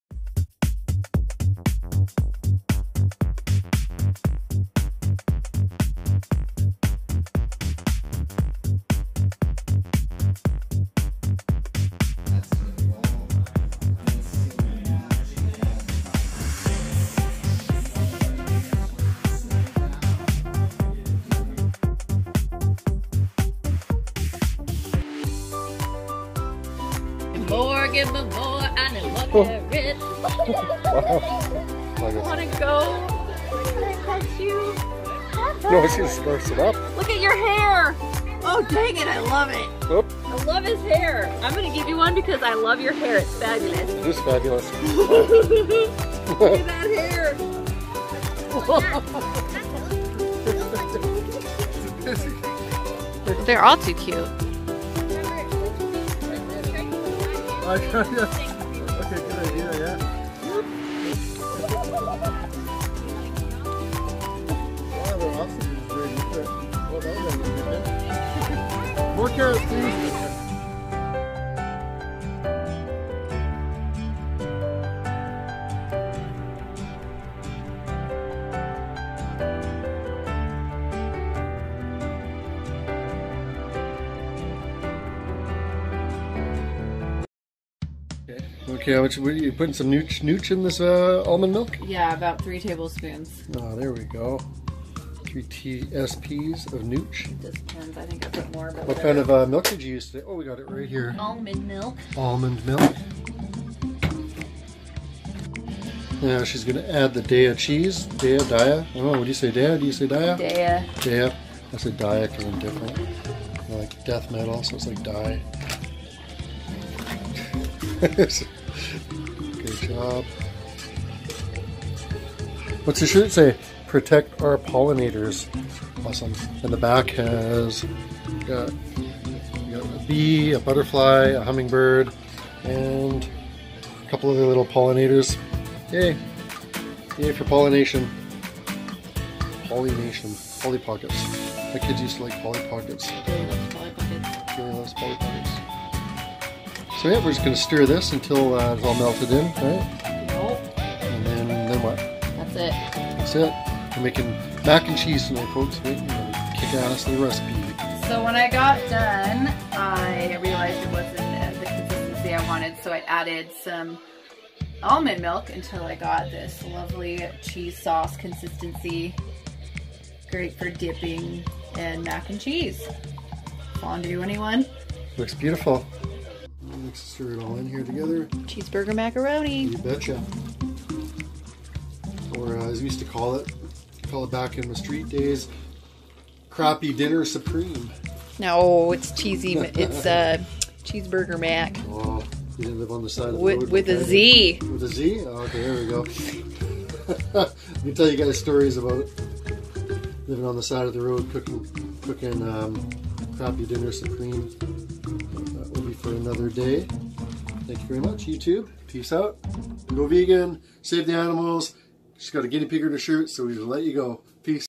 Our first weekend as a couple!
New vegan couple goes to animal sanctuaries and eats vegan food.